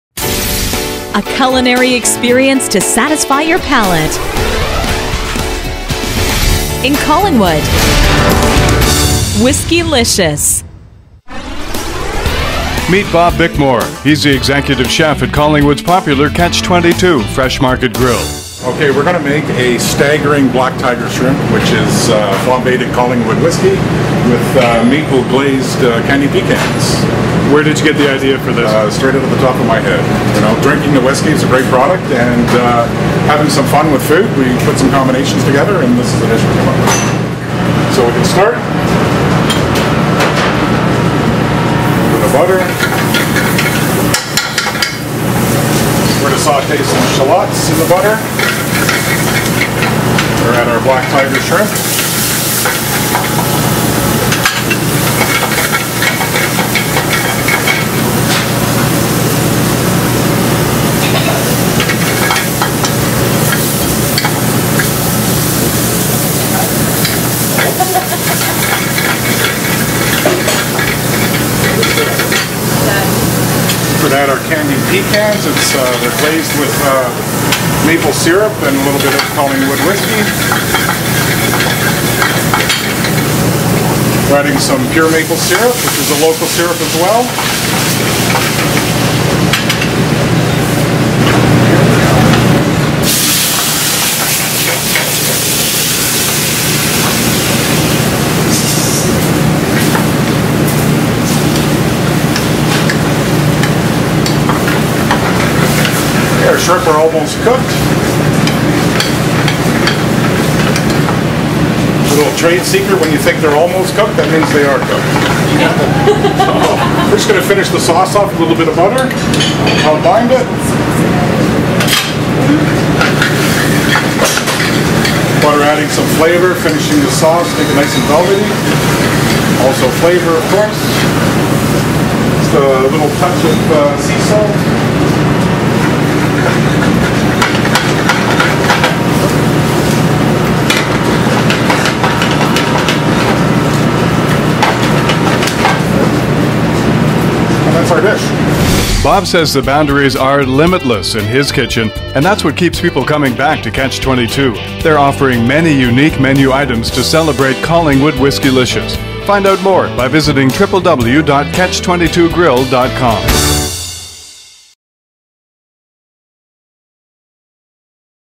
Catch 22 Fresh Market Grill - Collingwood Whiskeylicious
Bob Bickmore is the Executive Chef at Collingwood's Catch 22 Fresh Market Grill.  He is busy preparing special menu items to help celebrate Collingwood Whiskeylicious!